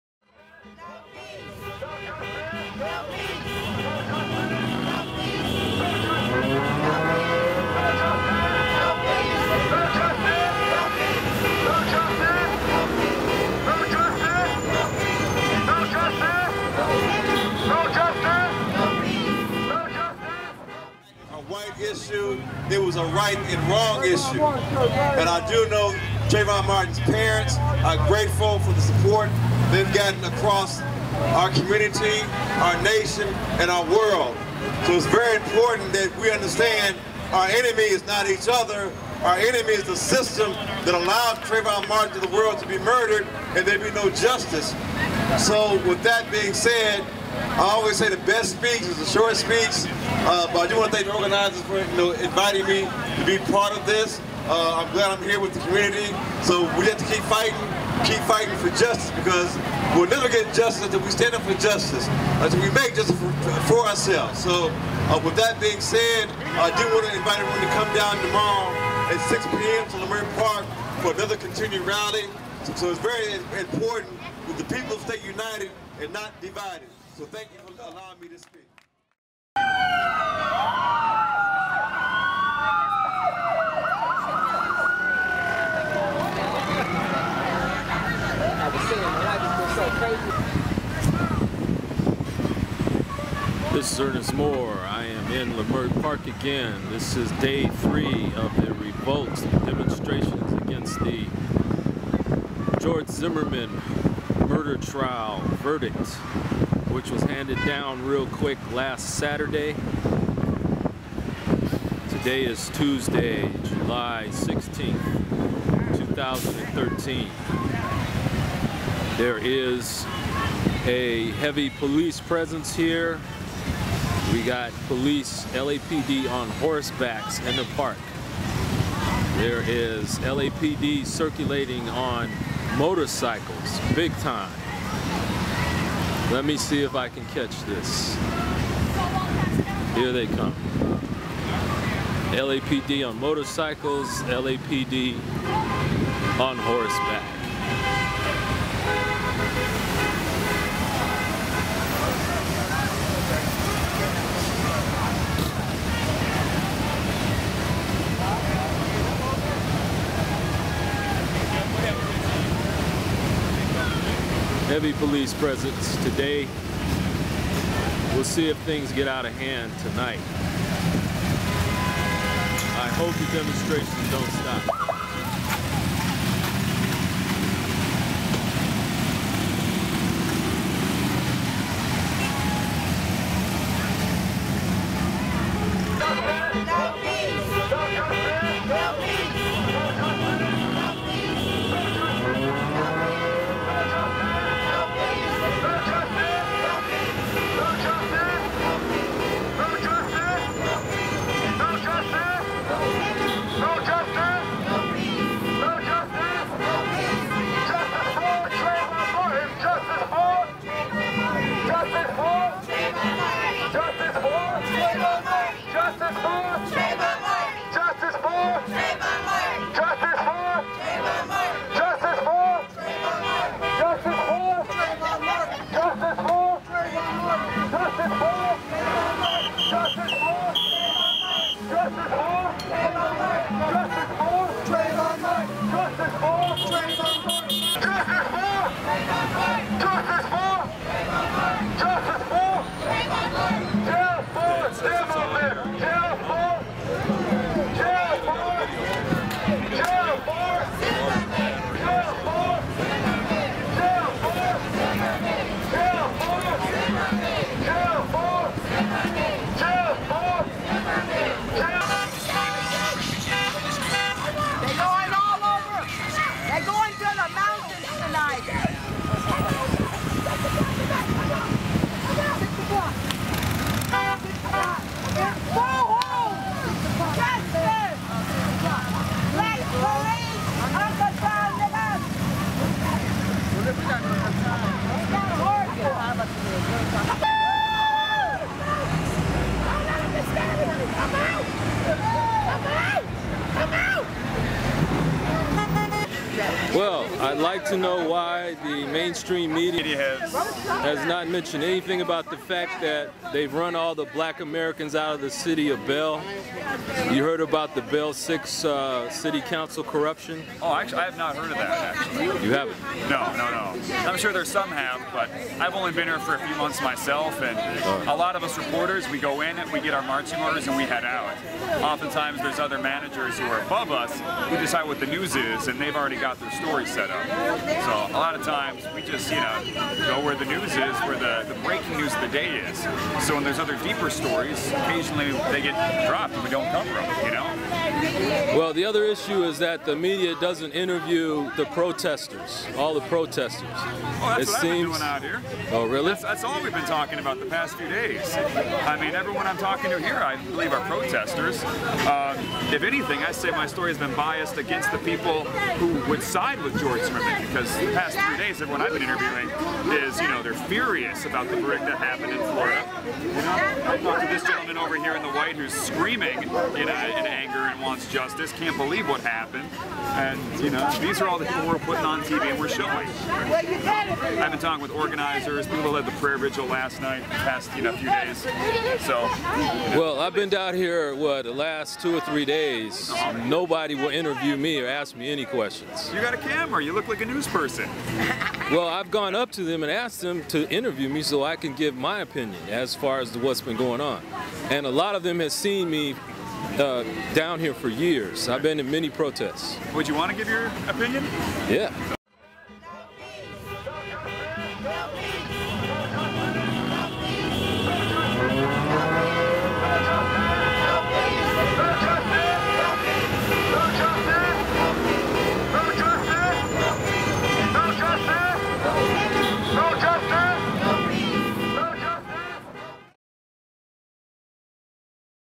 1st TJC Report on Zimmerman Verdict Revolts in Los Angeles
This video clip is the 1st special report on some of the demonstrations and revolts in Los Angeles, California as the result of the George Zimmerman verdict that came down on Saturday July 13, 2013. These events took place on Crenshaw Blvd and Martin Luther King blvd and at Crenshaw Blvd at Leimert Park in Los Angeles, California. All video and editing were done by Ernest L. Moore.